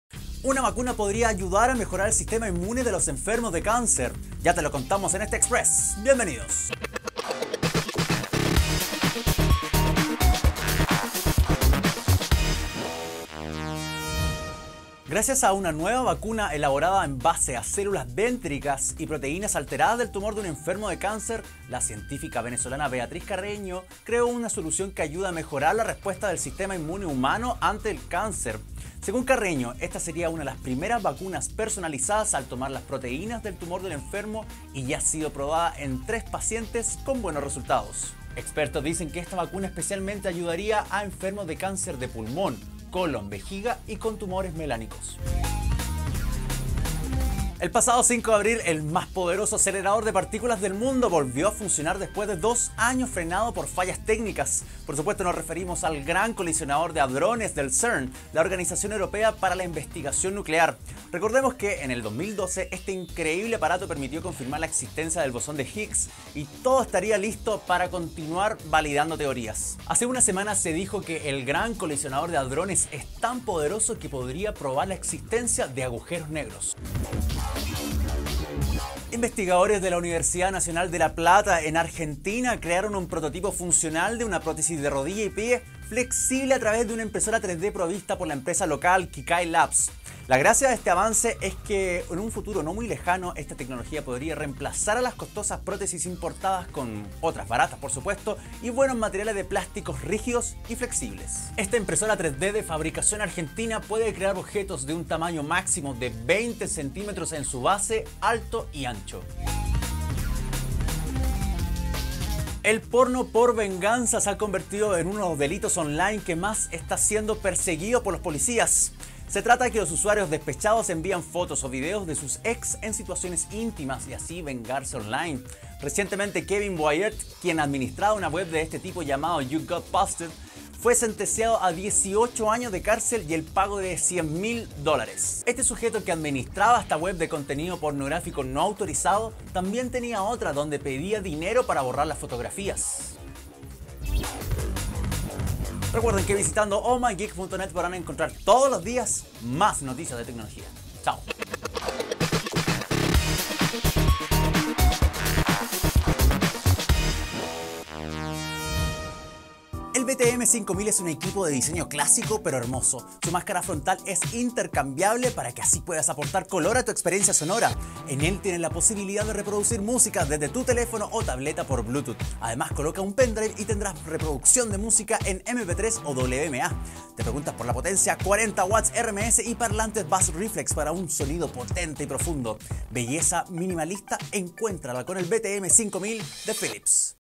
Vacuna de resistencia al cáncer, Gran colisionador de hadrones y Porno por venganza - OMG! Express
[00:14] La científica venezolana Beatriz Carreño creó un solución que ayuda a mejorar la respuesta del sistema inmune humano ante el cáncer.

[00:47] El pasado 5 de abril, el más poderoso acelerador de partículas del mundo, volvió a funcionar después de dos años.

[01:18] Investigadores de la Universidad Nacional de La Plata en Argentina crearon un prototipo funcional de prótesis de rodilla y pie flexible a través de una impresora 3D.

[01:51] El ‘porno por venganza’ se ha convertido en uno de los delitos que más está siendo perseguido por las distintas policías.

[02:39] Conoce un nuevo producto de Philips